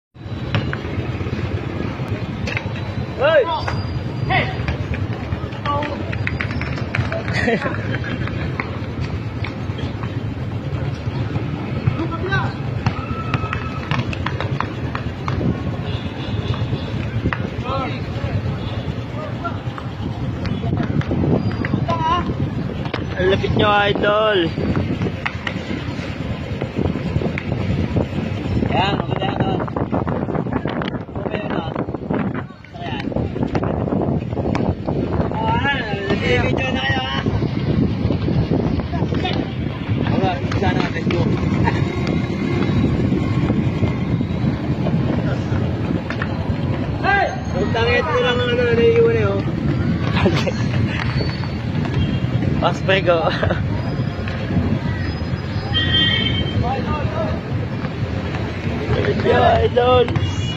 Pinoy Basketball Court
tuloy pa rin ang laro kahit may naka park na sasakyan sa gitna ng court